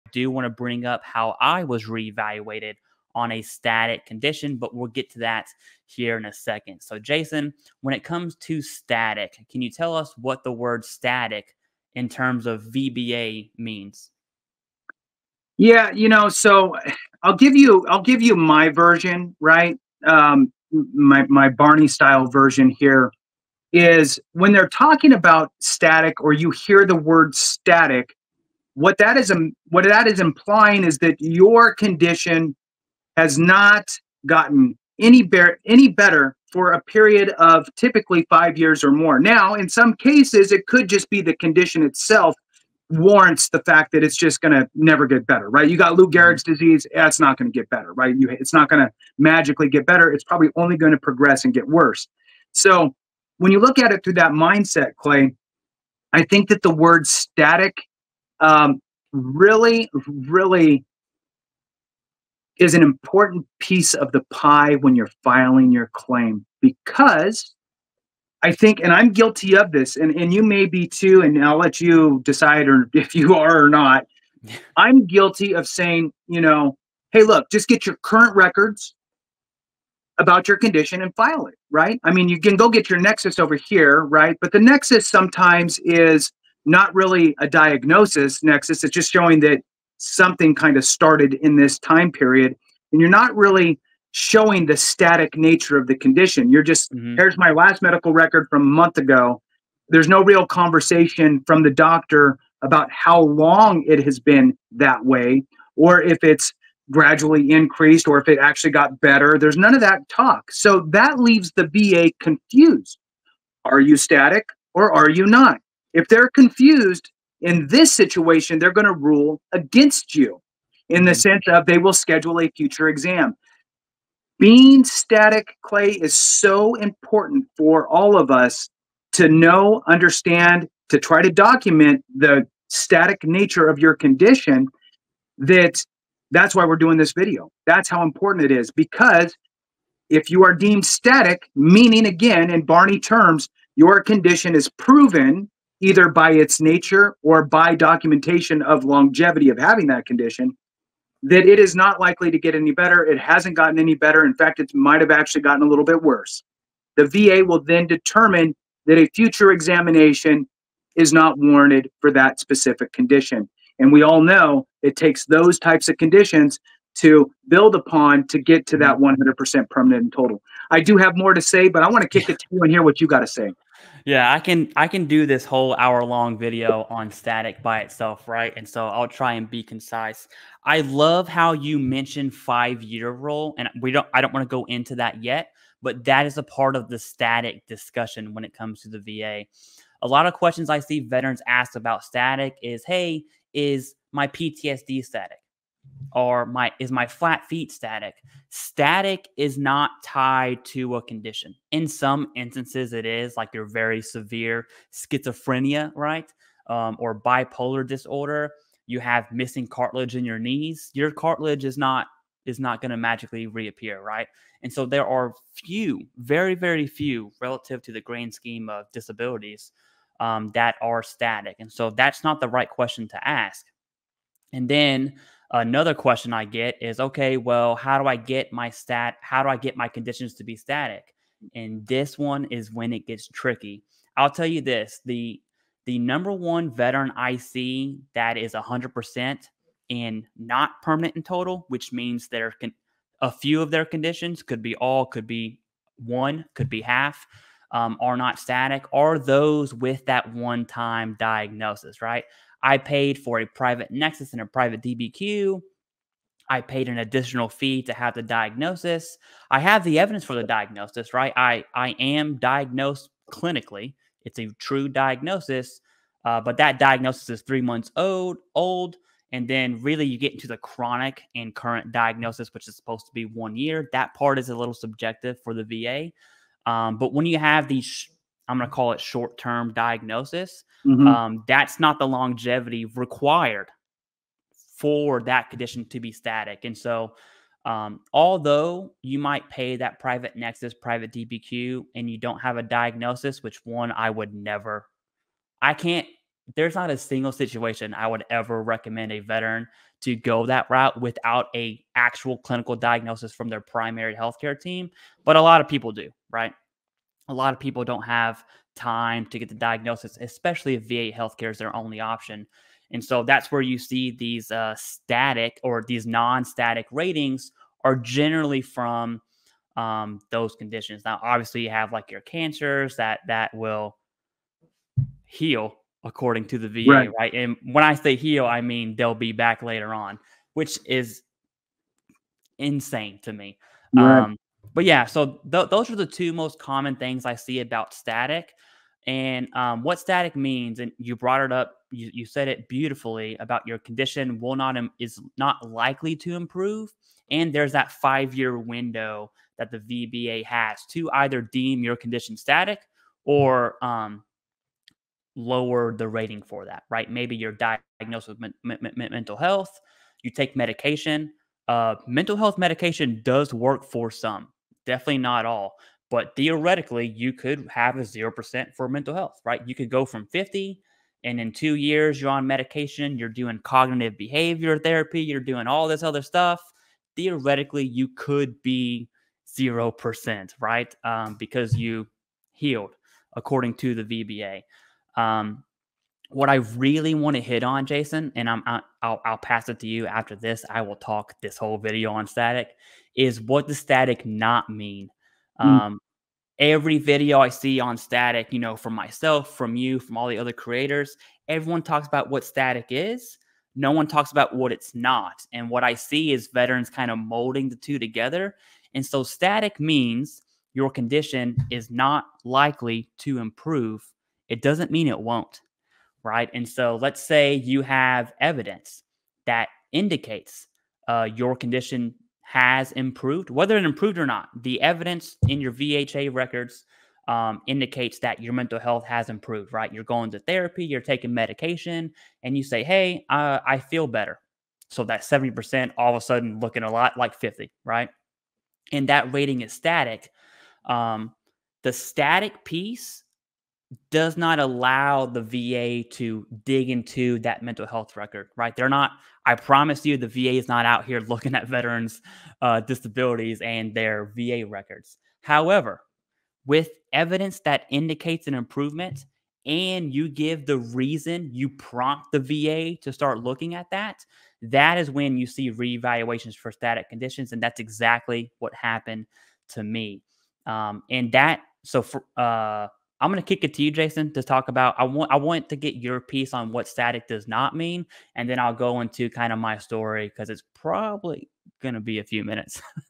STATIC VA Disability Explained And Why It Matters For Veterans And VA Ratings
Veterans Info Tap and TheCivDiv discuss what a static VA disability rating is and what it is not, as well as the 5, 10, 20 and 55yr rule.